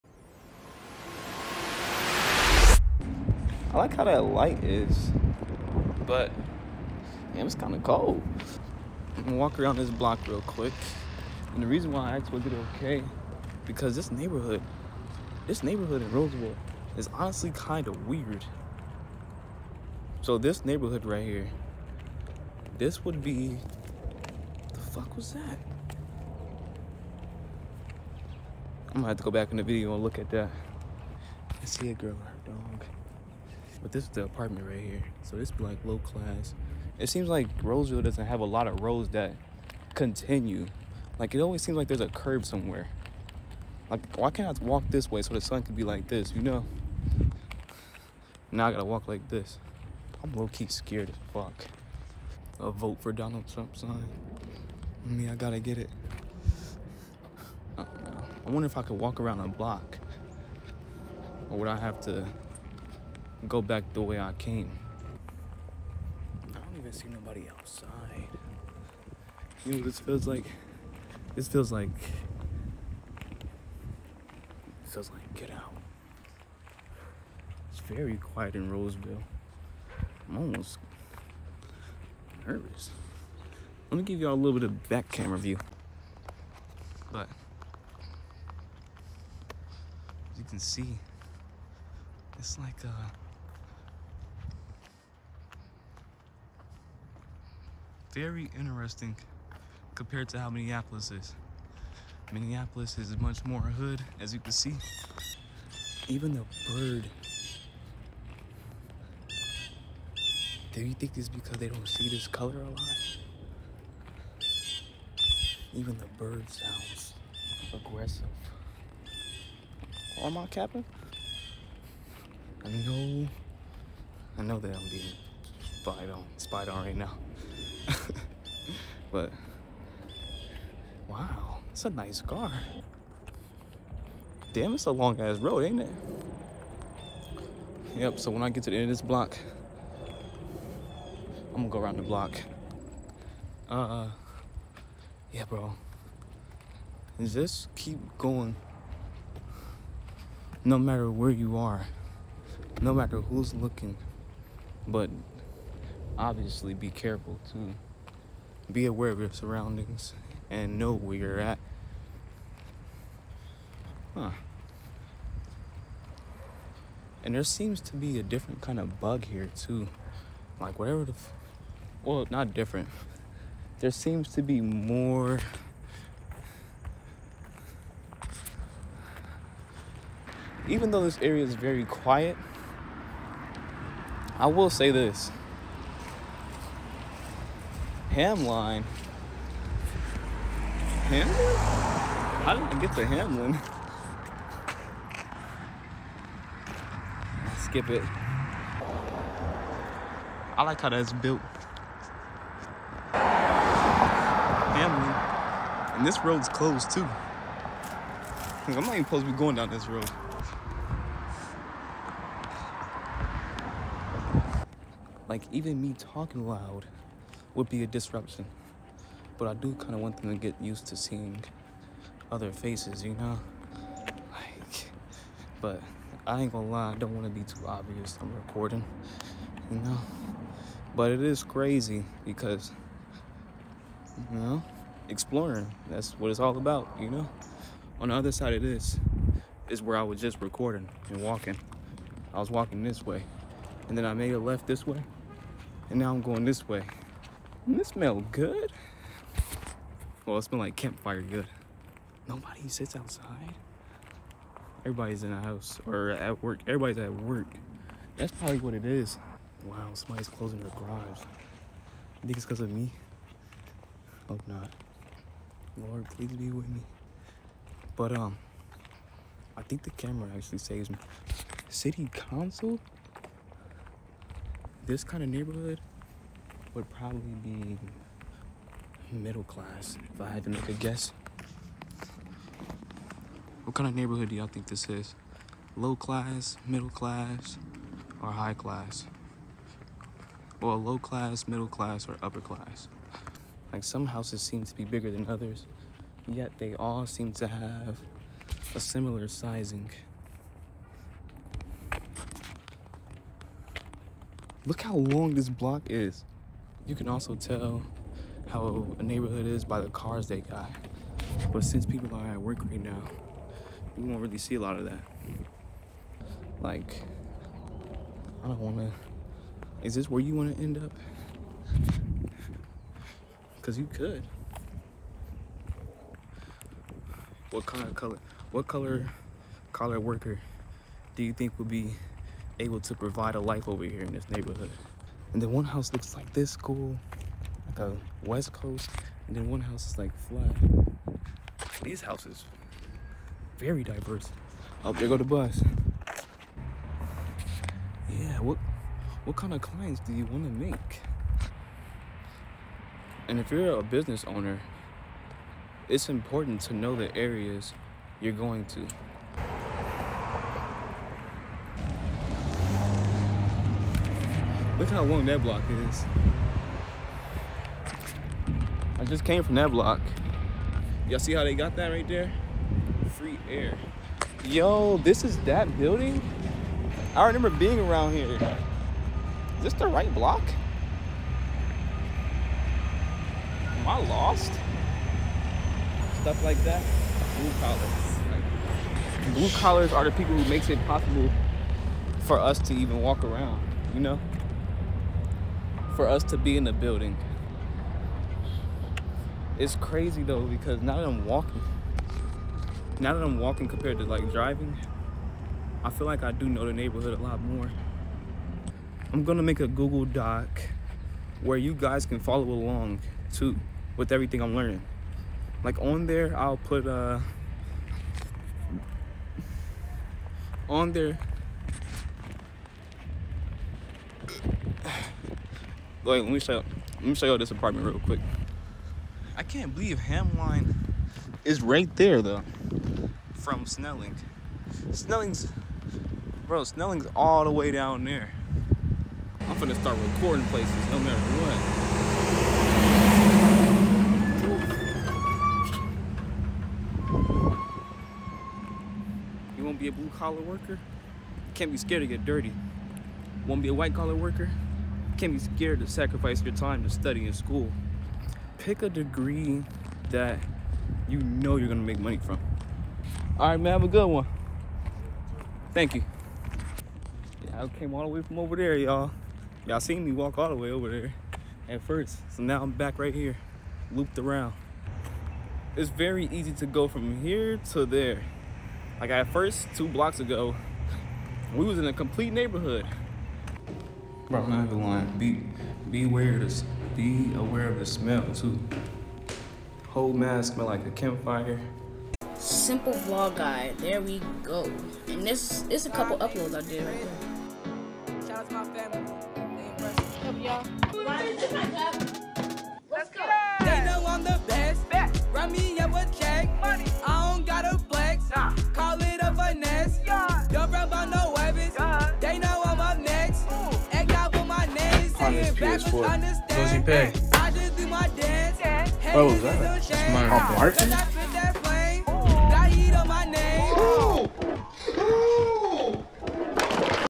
Get out your comfort zone! (In the Suburbs alone) Do not try this at home!😭)
Do things that are uncomfortable and hard. The more you do them, the more confident and comfortable you will become.But obviously be careful too😭